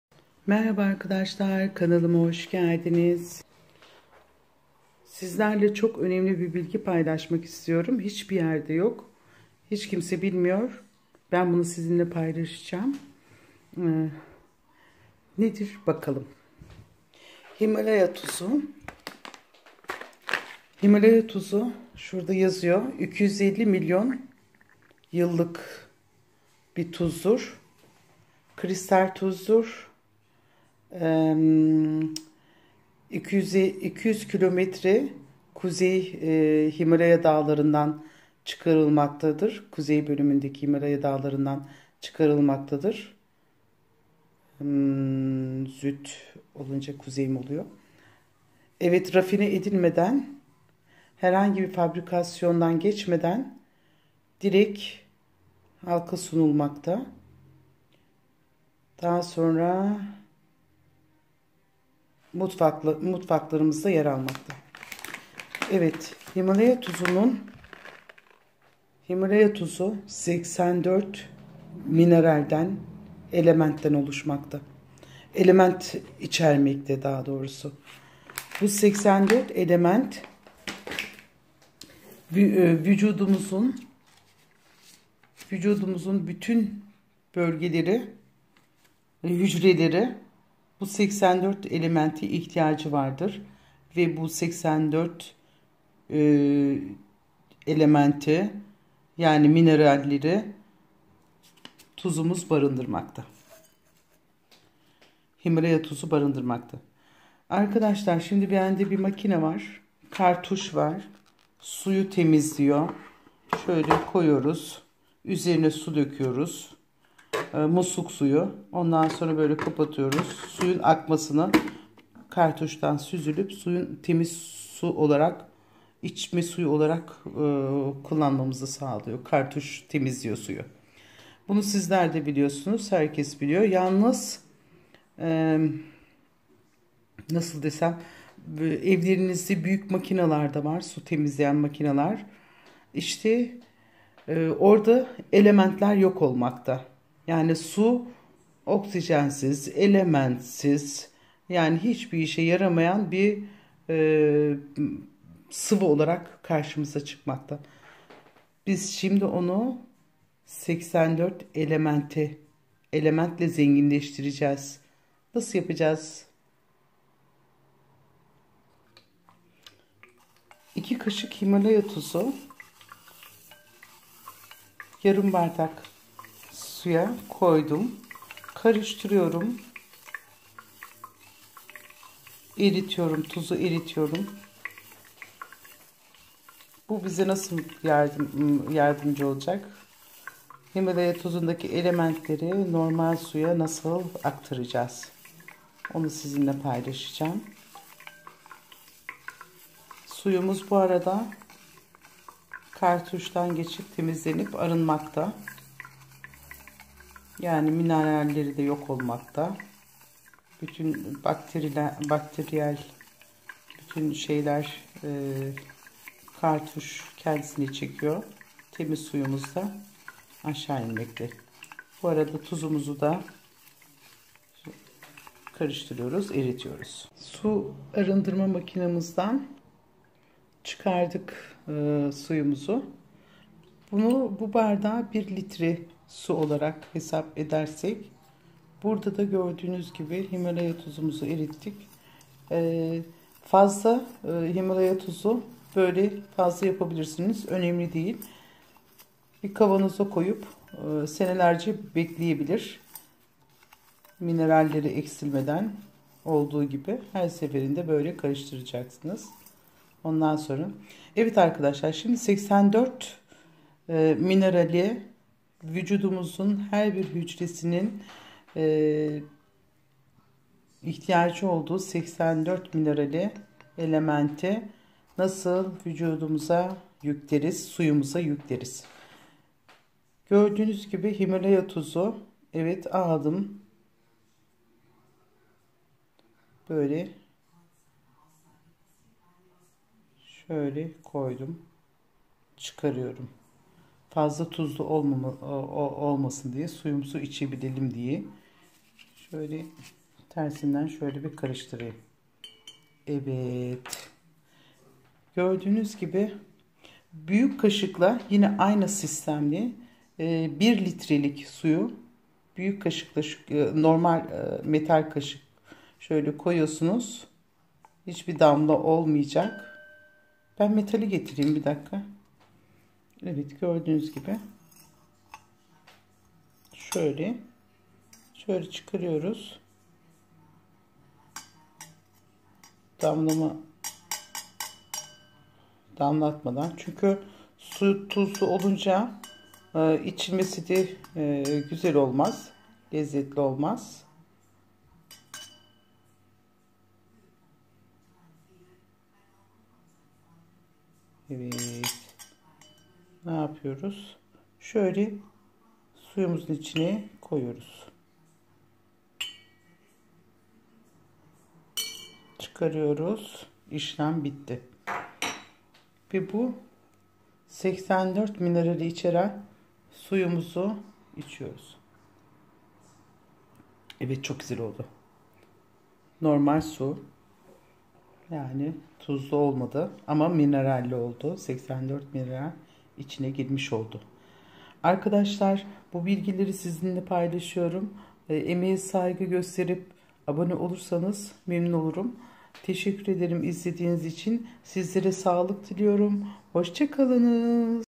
SU YA MiNERAL YÜKLEME👏 PIRLANTA DAN DAHA DEGERLi BIR SIRRI SiZLERE SUNUYORUM.😮
evlerinize aldiginiz temiz su makinalarindan cikan suyun hic bir saglik yönünden faydasi yoktur, bütün  saglikli mineraller sudan arindirilmis  ve oksijensiz  bir suyu icmis olacaksiniz, peki bunu önlemek ve suya mineral yükleyebilirmiyiz...

EMEGE SAYGI